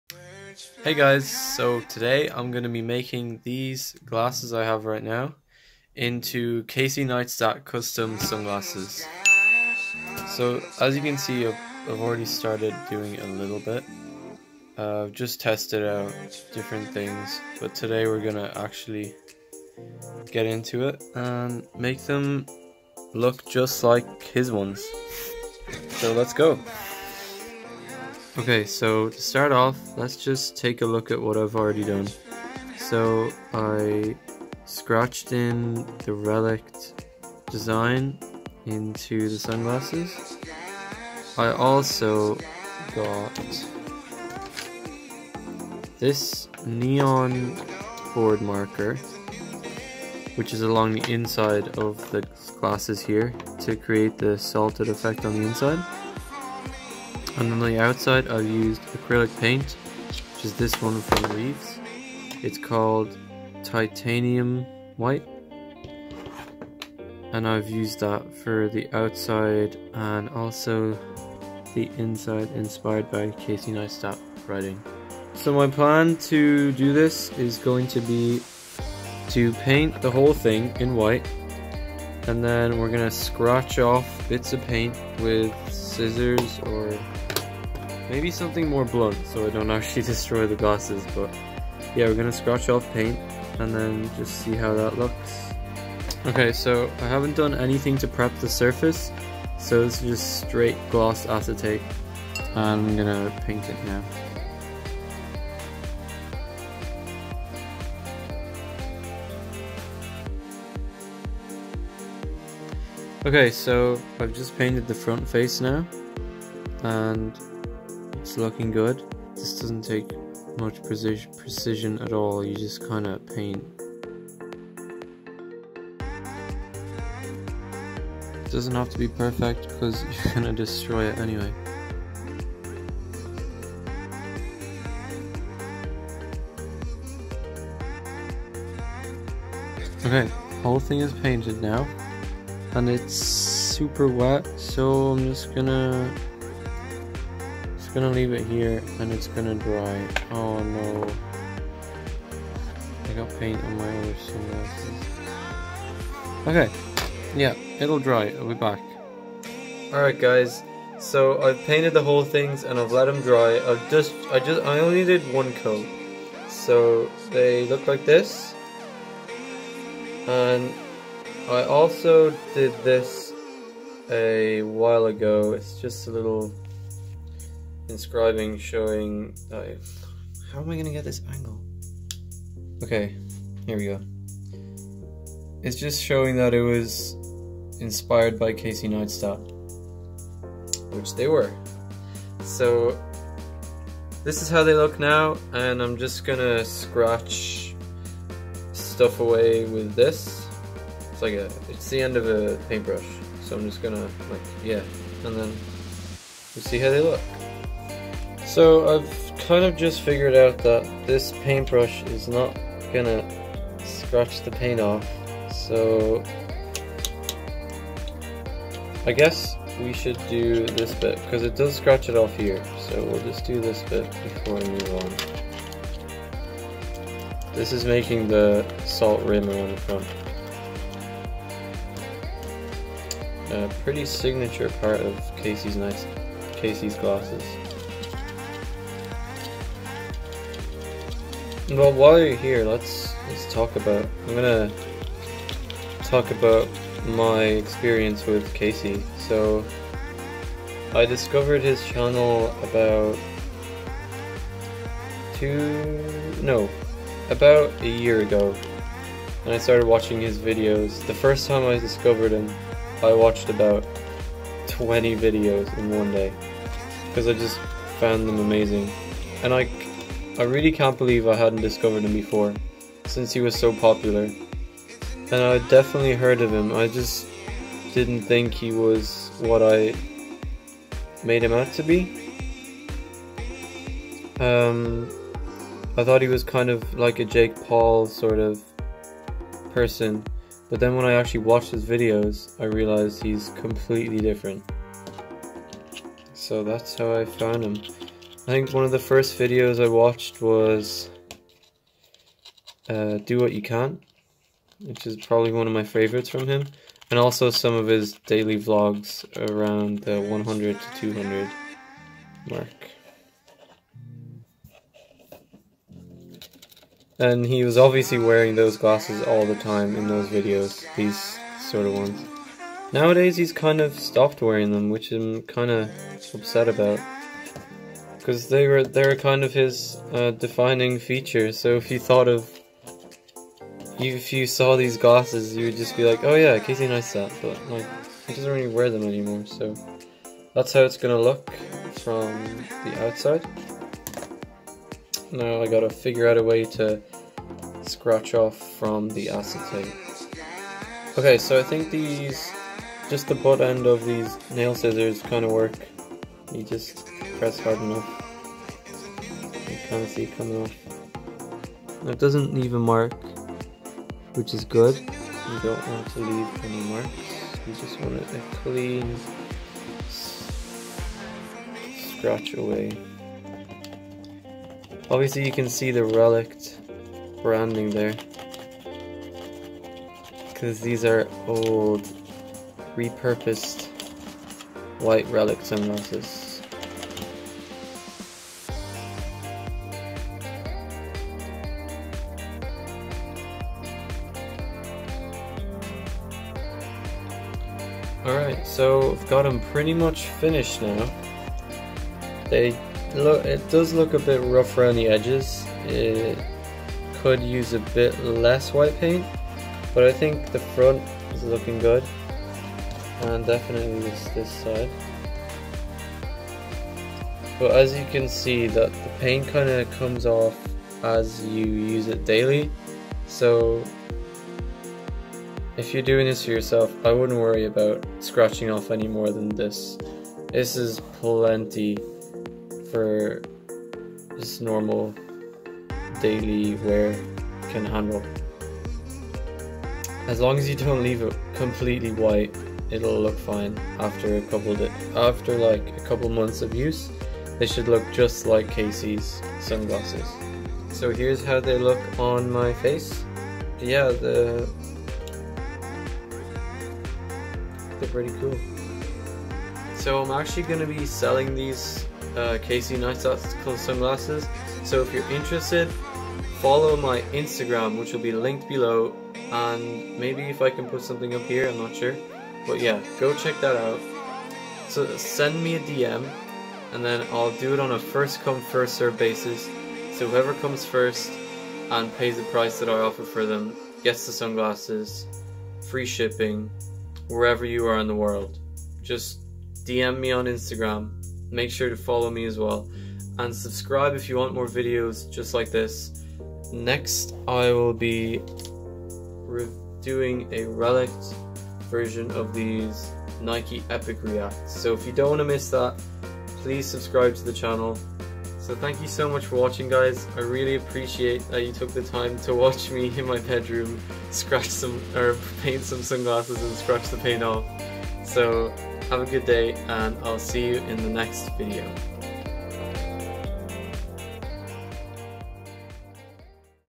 MAKING CASEY NEISTAT CUSTOM SUNGLASSES - Step by Step tutorial for making these custom sunglasses
In this video I show you how I make Custom Casey Neistat sunglasses so that they look just like the ones he makes! Of course if you want to watch casey's video its